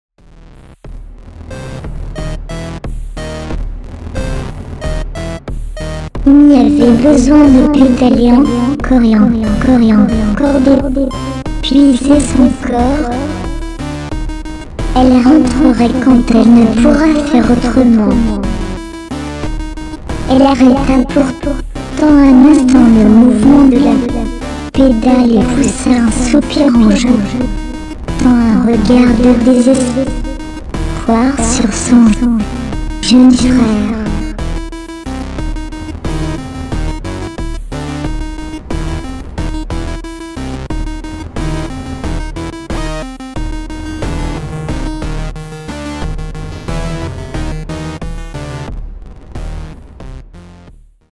Micro-événement (Saison 1, Épisode 3)
Paul Méphisto, séduit par la brièveté des micro-événements, se lance à son tour  et propose avec cet épisode 3 de la saison 1 sa treizième vidéosérie d'Un Monde Incertain.